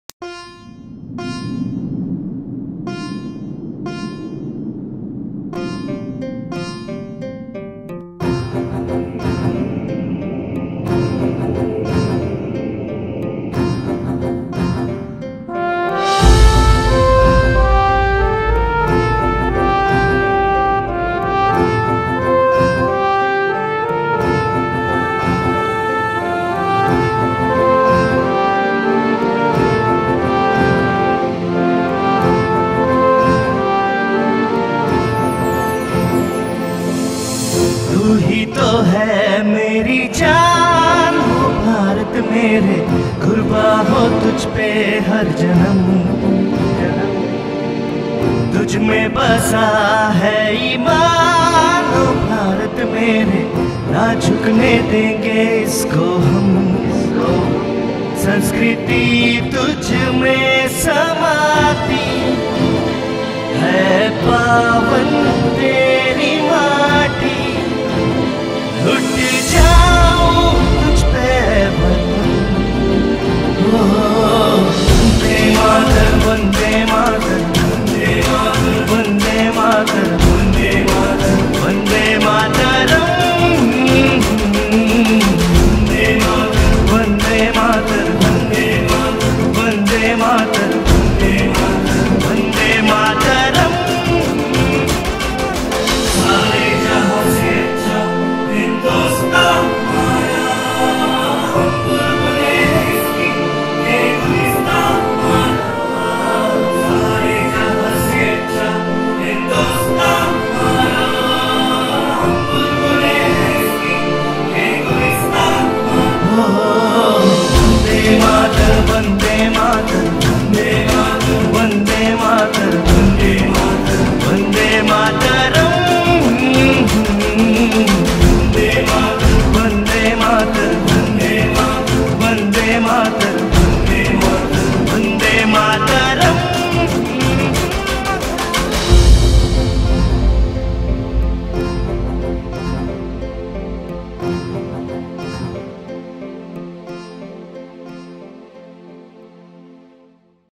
71st Independence Day Celebrations🎈🎈🎶🎶
On 15 August 2017, India celebrates her 71st Independence Day.